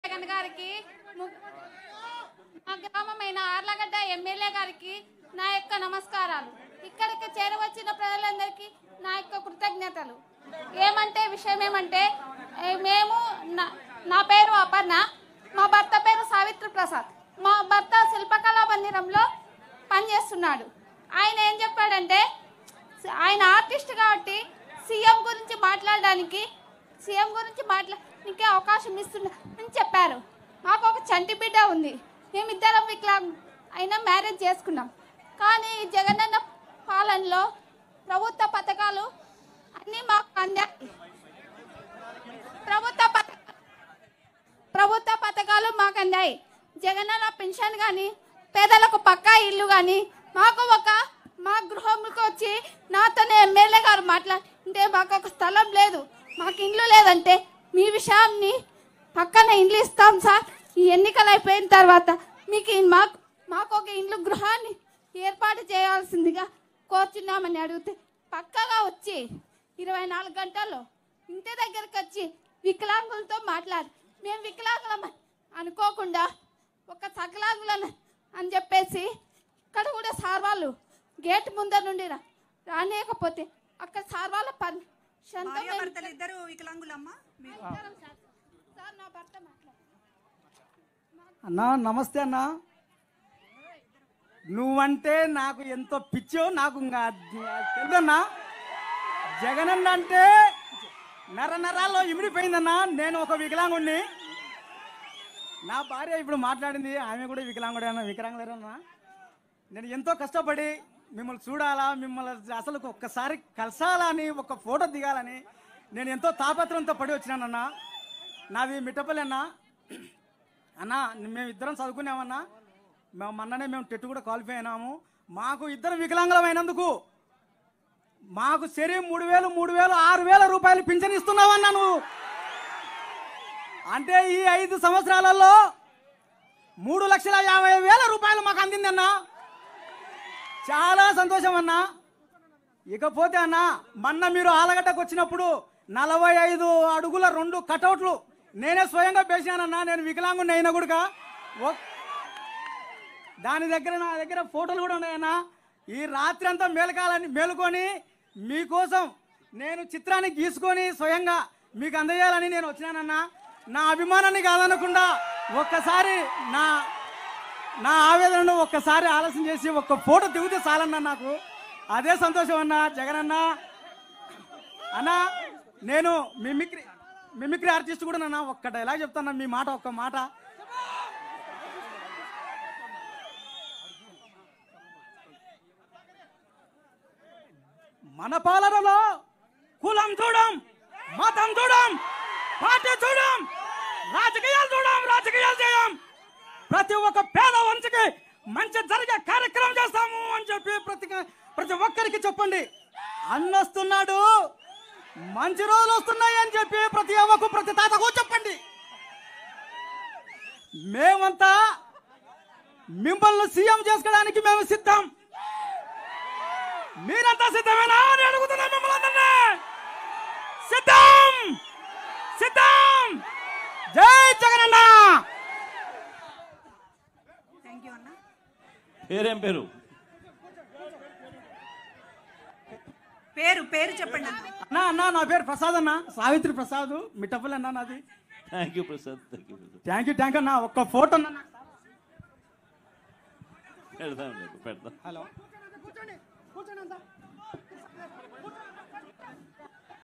సీఎం జగన్ బొమ్మ గీసిన వికలాంగుడు |Yerraguntla Public Meeting | CM Jagan Bus Yatra | Telugu Rajyam
ఆళ్లగడ్డ ప్రజలతో జగనన్న ముఖాముఖి | 🔴CM Jagan LIVE : Memantha Siddham Public Meeting @ Allagadda
 Memantha Siddham Yatra - Day 1 | YSJaganAgain | YSRCP | Telugu Rajyam